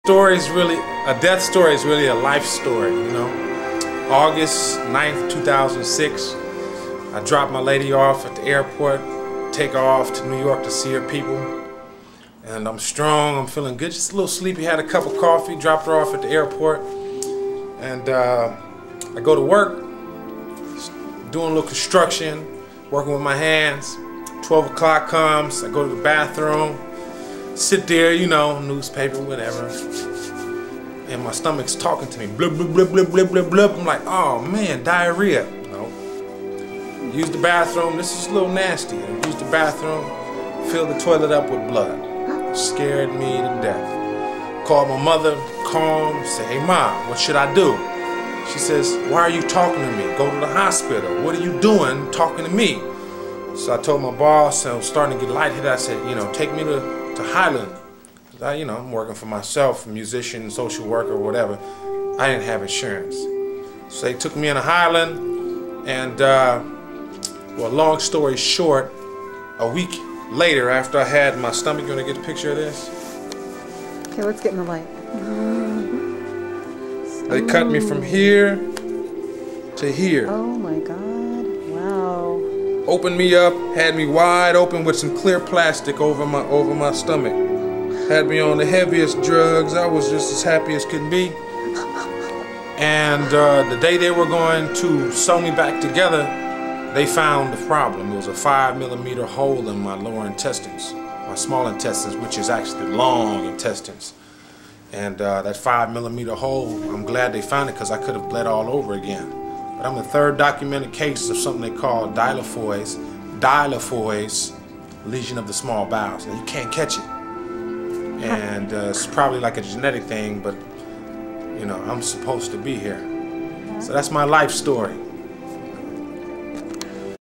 DEATH & SCARS: Atiim Bomani Chenzira
ATIIM CHINZERA BOMANI shares his near death story and his amazing two foot long gut scar